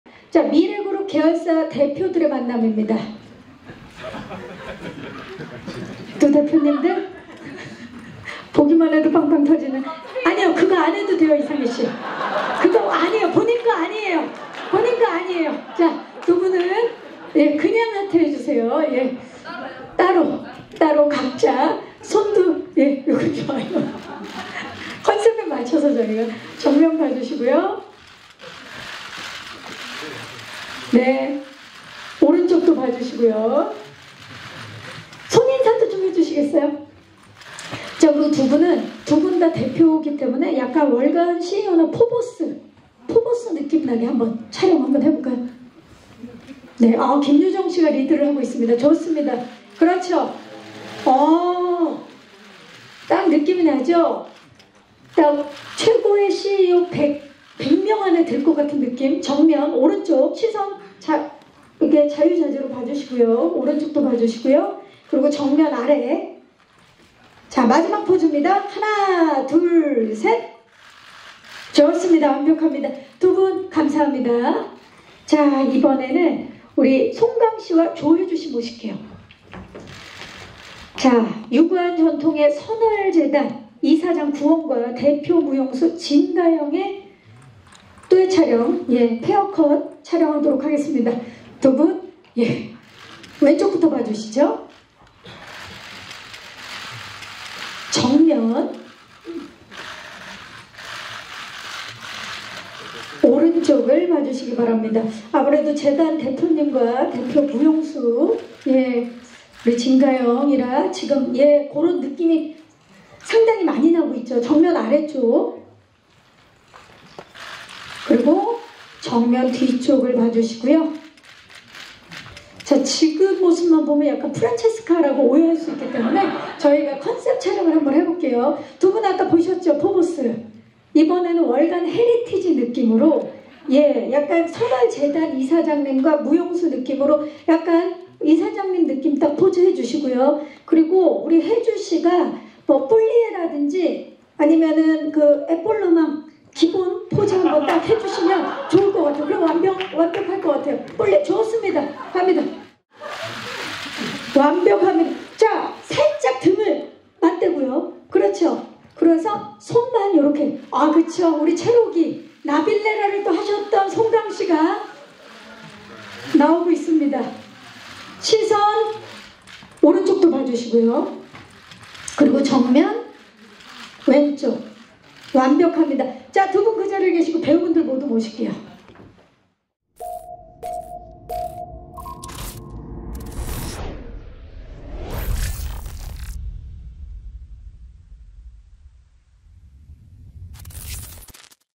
이상이X김유정X송강X조혜주, ‘마이 데몬’ 재벌과 악마의 복잡 미묘한 관계♥
24일 오후 서울 양천구 목동 SBS 사옥에서 SBS 금토드라마 '마이 데몬' 제작발표회가 열렸다.

이날 ‘마이 데몬’에 출연하는 배우 김유정, 송강, 이상이, 조혜주와 김장한 감독이 참석했다.

‘마이 데몬’은 재벌 상속녀 도도희(김유정 분)와 한순간 능력을 잃어버린 ‘악마’ 정구원(송강 분)이 계약 결혼을 하며 벌어지는 이야기를 다룬다.